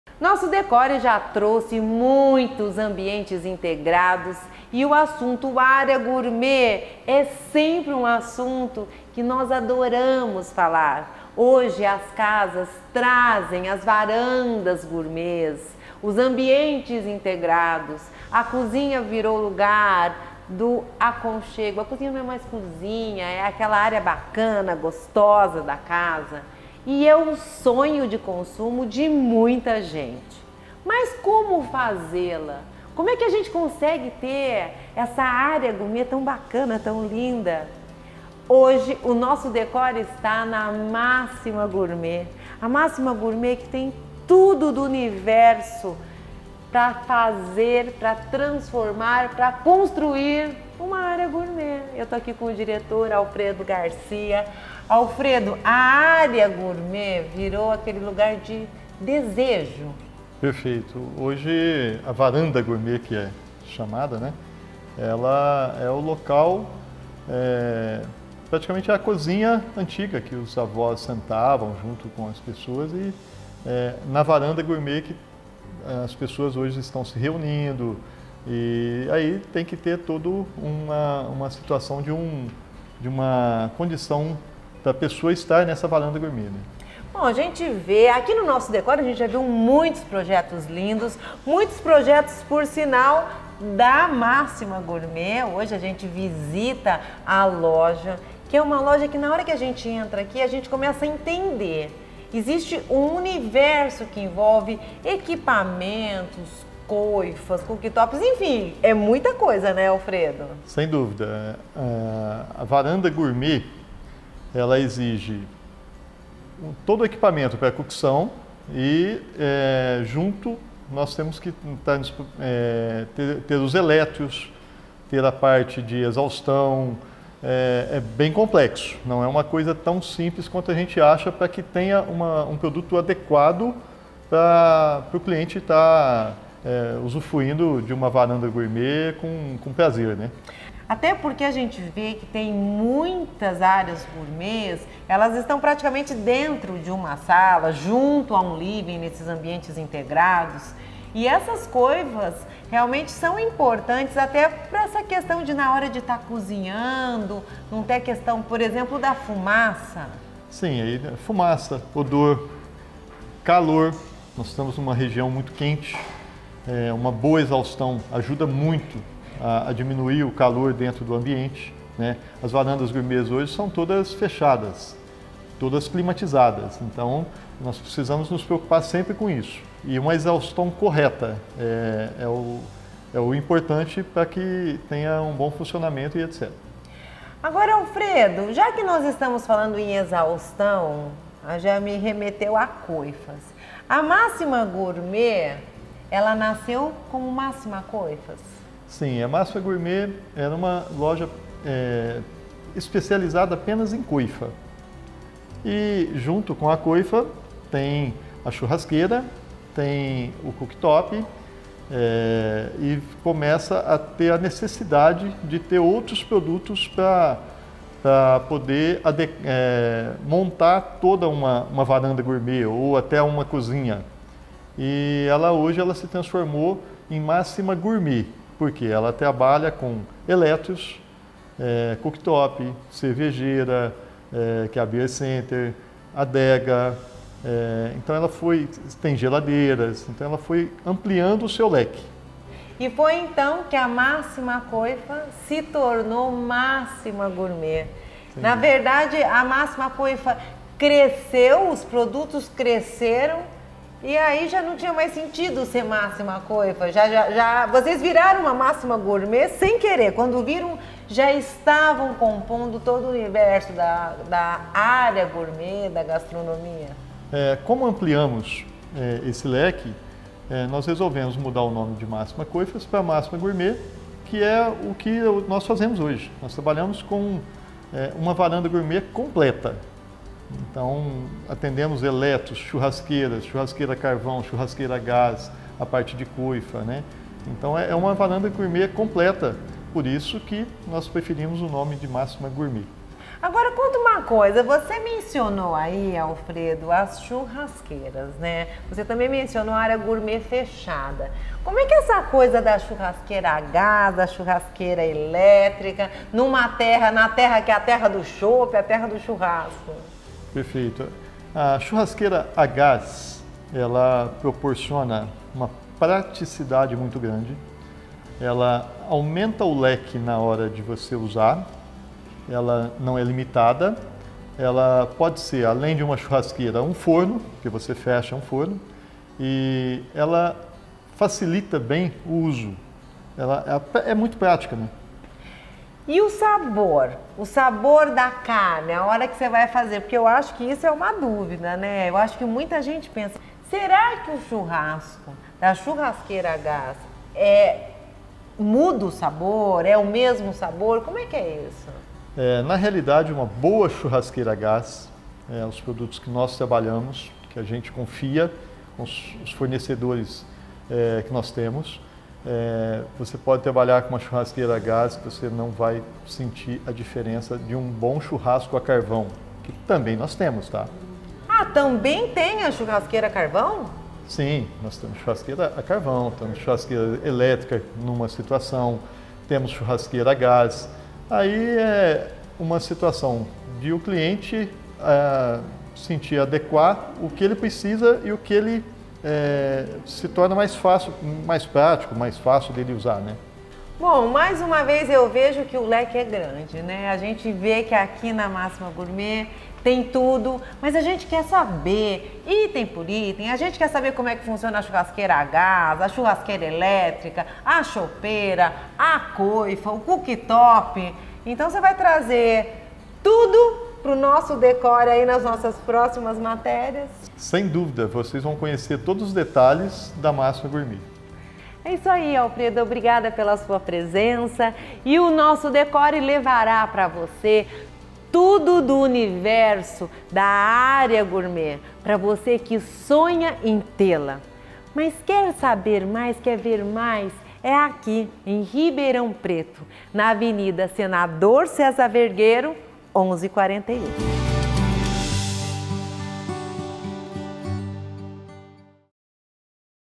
A área gourmet dos seus sonhos em um Ambiente Integrado | Lelinha Gentil
Como ter uma área gourmet bacana na sua casa? É isso que iremos descobrir hoje com Alfredo Garcia, diretor da máxima gourmet em Ribeirão Preto!